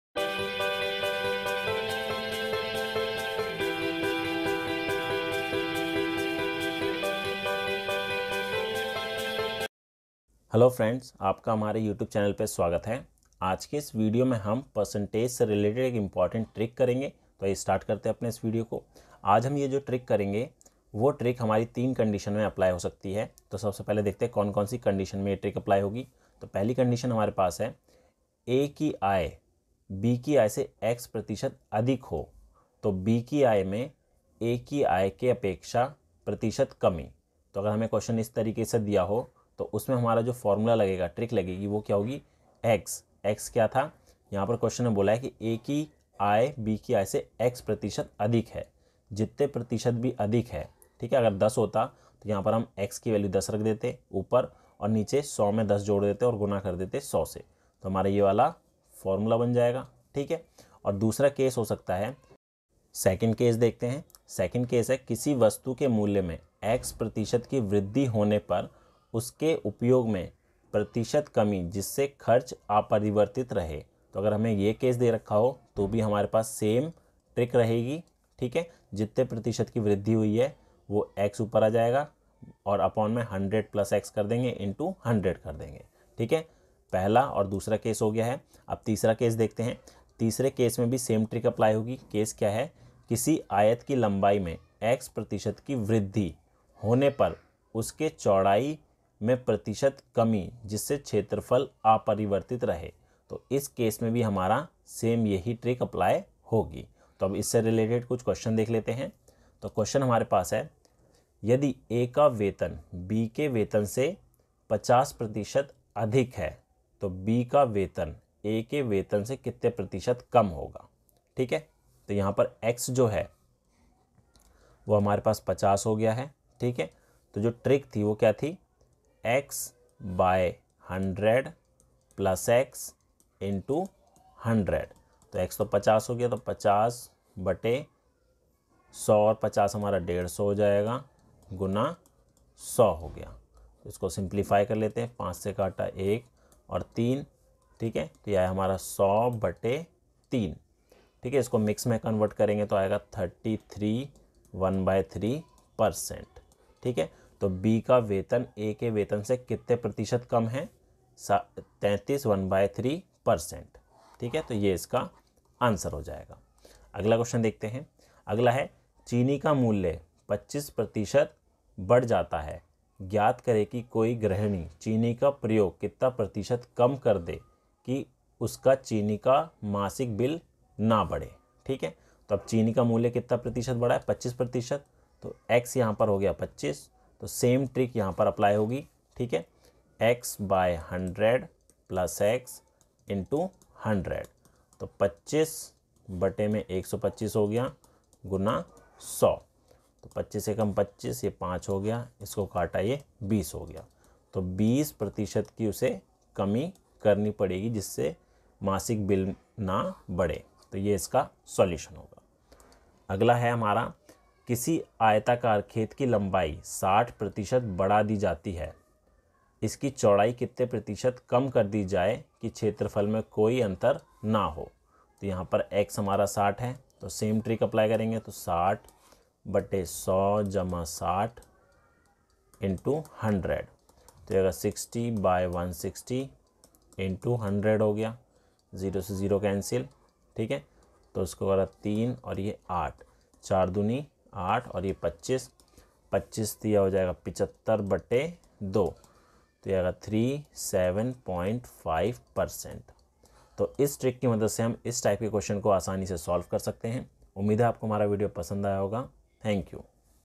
Percentage Short (Trick-1) || प्रतिशत Tricks
Example 1 यदि A का वेतन B के वेतन से 50% अधिक है, तो B का वेतन A के वेतन से कितना प्रतिशत कम है?

Example - 2 चीनी का मूल्य 25% बढ़ जाता है. ज्ञात करें कि कोई गृहीणी चीनी का प्रयोग कितना प्रतिशत कम कर दे, कि उसका चीनी का

Example - 3 किसी आयताकार खेत की लम्बाई 60% बढ़ा दी जाती है.इसकी चौड़ाई कितना प्रतिशत कम कर दी जाए कि क्षेत्रफल में कोई अन्तर न हो ?